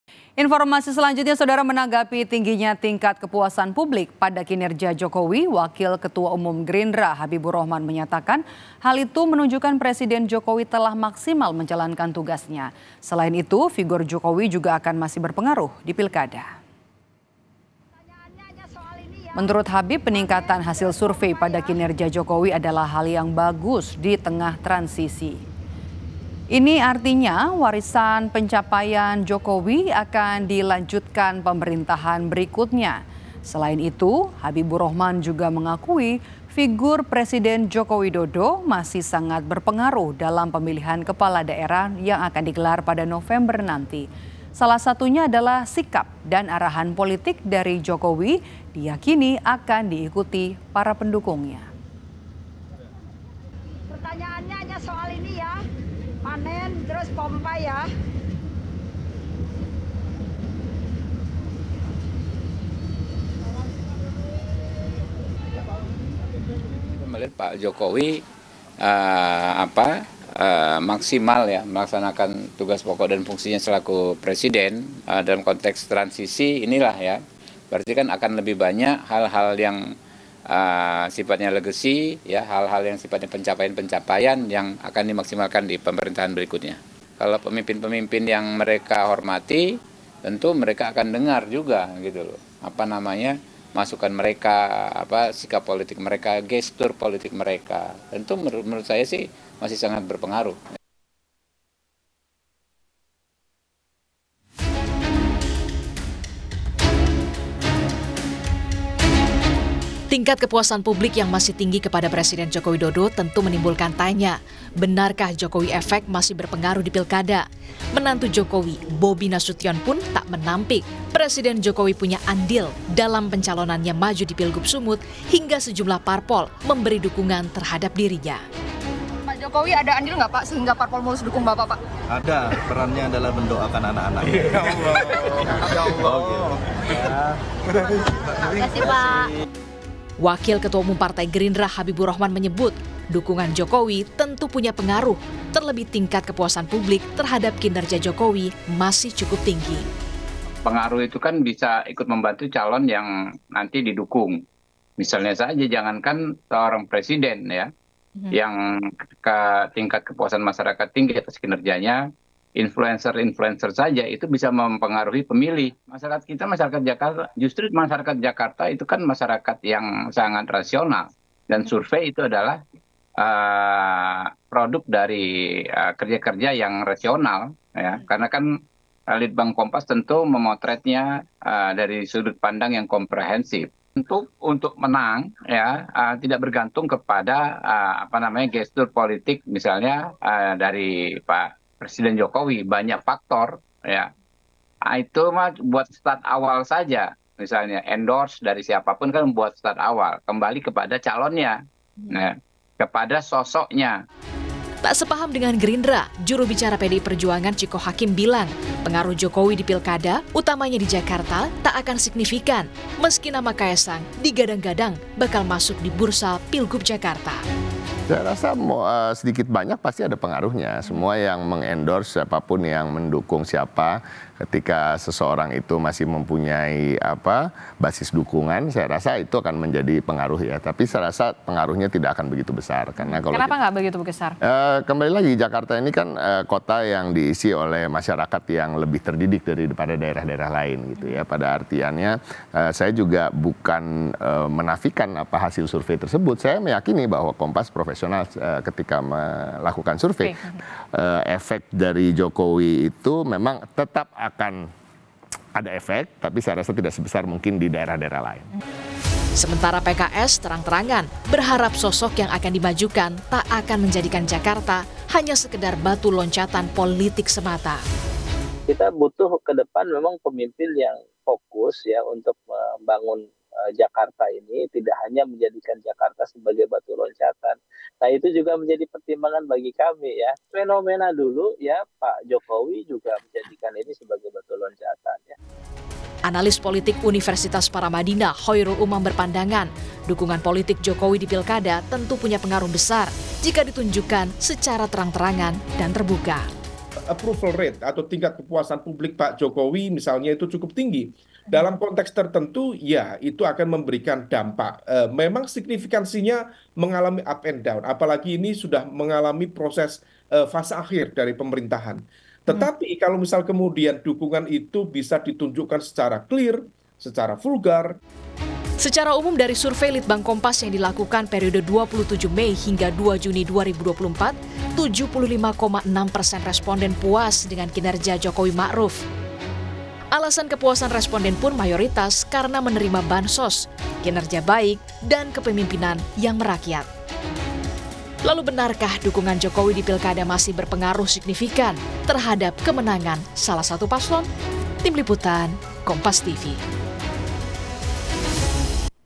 Alasan Gerindra Yakin Jokowi Masih Berpengaruh di Pilkada Serentak
KOMPAS.TV - Menanggapi tingginya tingkat kepuasan publik pada kinerja Jokowi, Wakil Ketua Umum Gerindra, Habiburokhman menyatakan hal tersebut menunjukkan Presiden Jokowi telah maksimal menjalankan tugasnya.

Selain itu, figur Jokowi akan masih berpengaruh di Pilkada.

Menurut Habib, peningkatan hasil survei pada kinerja Jokowi, adalah hal yang bagus di tengah masa transisi. Ini artinya, warisan pencapaian Jokowi akan dilanjutkan pemerintahan berikutnya.

Baca Juga Alasan PDI-P Lirik Susi Pudjiastuti Maju di Pilkada Jawa Barat di